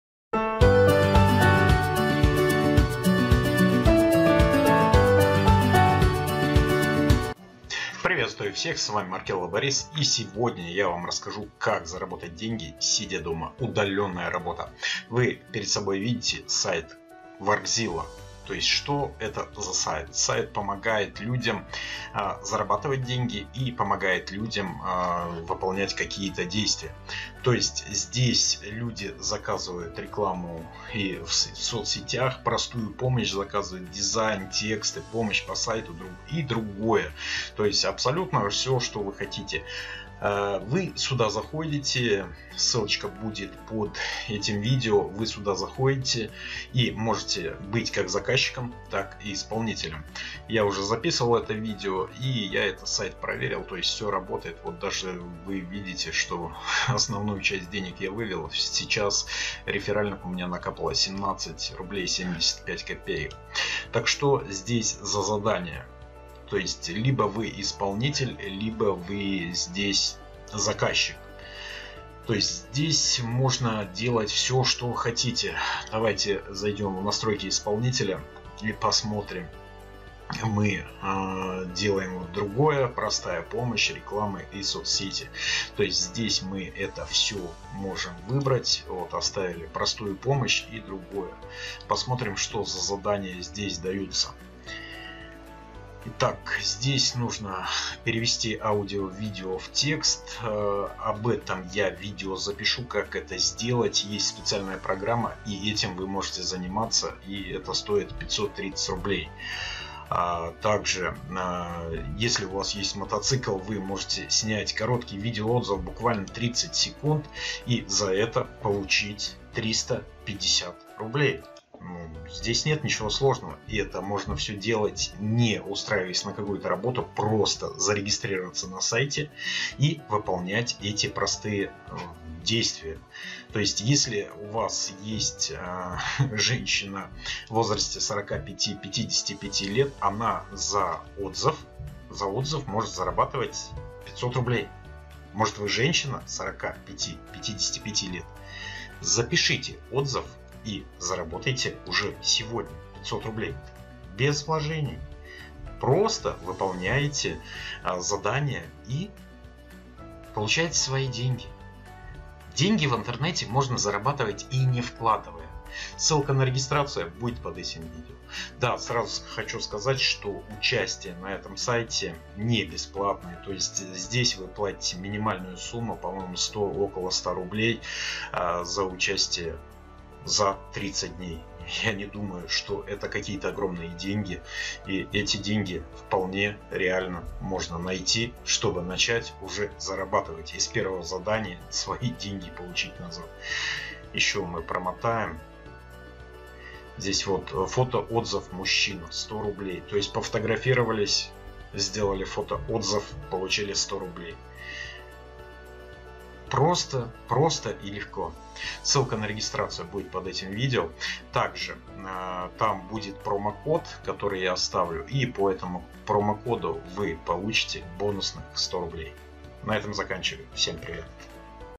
Как заработать деньги сидя дома. Удаленная работа на дому. Как заработать в интернете.
Здравствуйте, дорогие друзья. Многим стала интересна удаленная работа. Поэтому сегодня мы рассмотрим как заработать в интернете. Это даже можно сказать заработок в интернете 2019. Работа в интернете на дому доступна всем и каждому. Нужен только компьютер и выход в интернет.